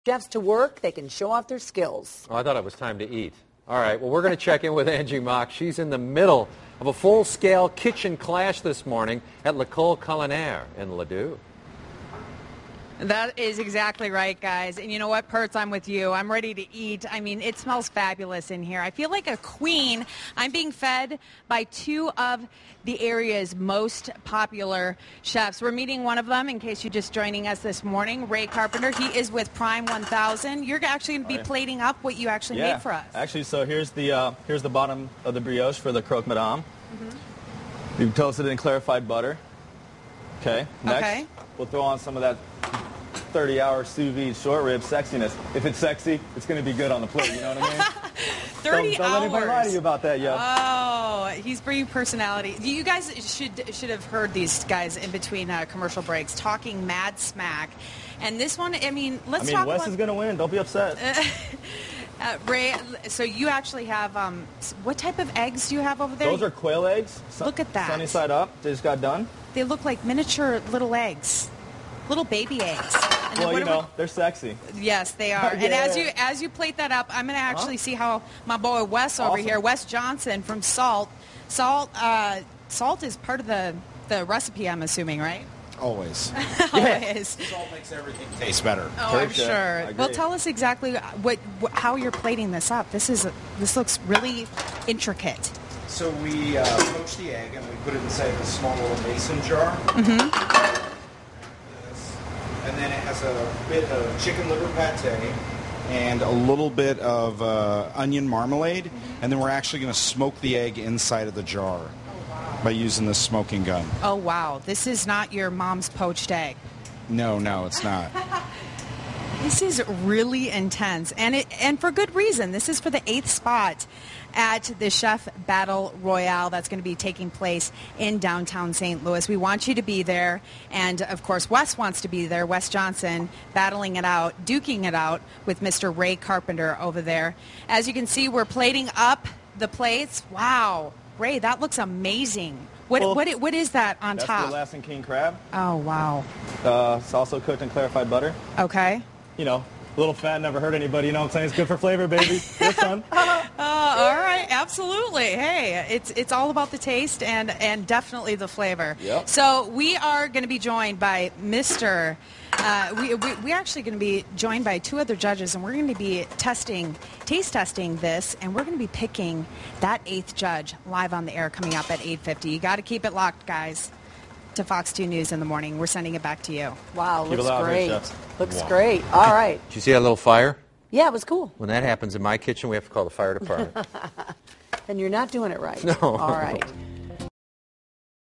Chefs Battle For Spot In Taste Of St. Louis
A culinary campaign got underway Monday morning with FOX2's Angie Mock and News 11's Patrick Clark. There was no bloodshed at breakfast, but the two sat down to judge a battle royale amongst area chefs. The winner gets a berth in this weekend's Taste of St. Louis Food Competition. As Patrick says,